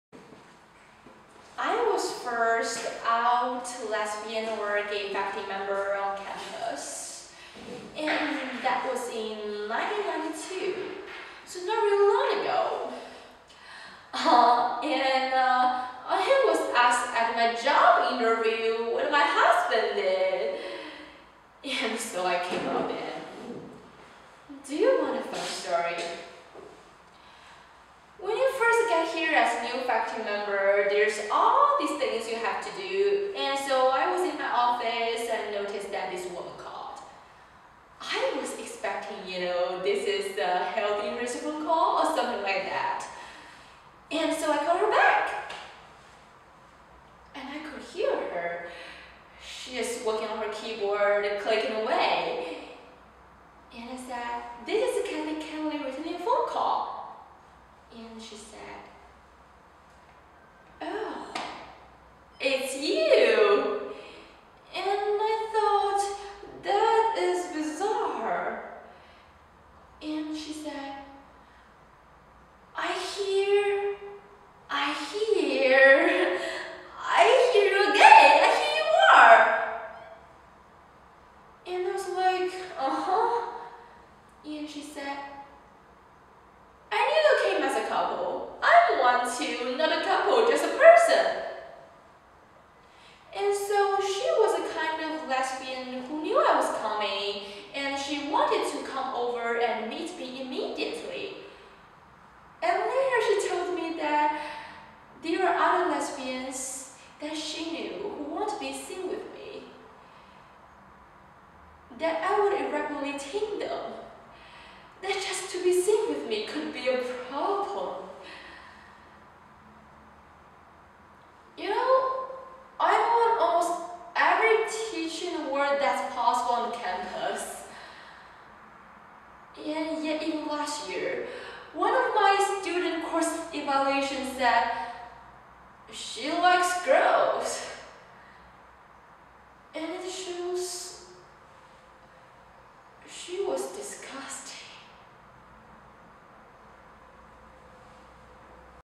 Monologue “The Laramie Project”
Monologue “The Laramie Project”, performed by Claire Emerson College Video Audition